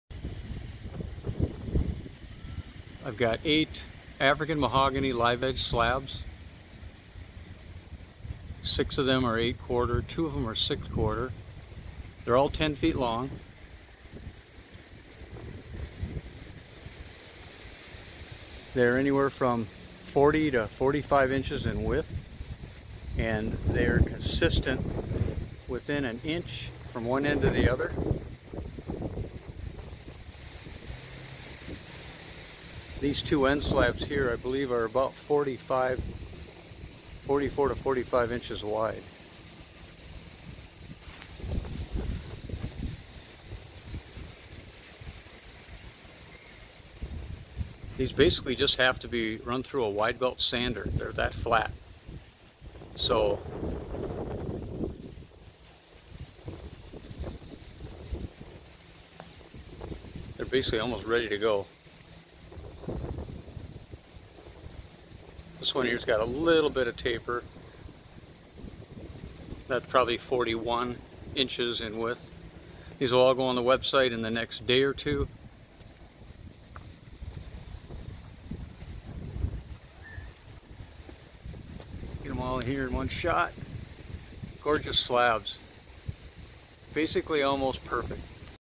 African mahogany Live Edge slabs just added to website
These beautiful huge African mahogany Live Edge slabs are now available online. Logs-to-Lumber.Com